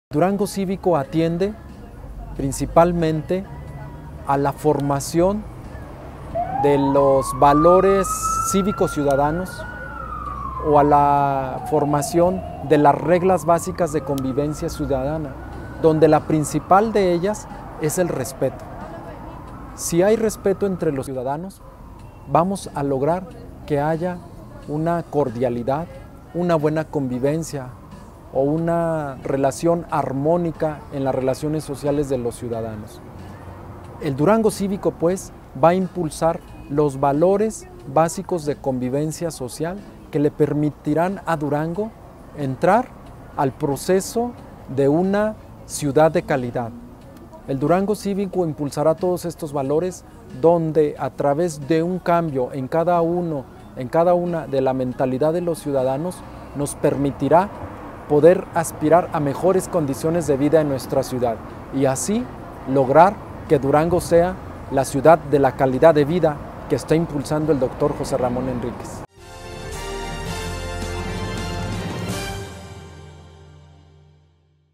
Durango Cívico pretende armonizar conflictos
La Mediación ciudadana provocará que tengamos un Durango Cívico: Noel Flores Reyes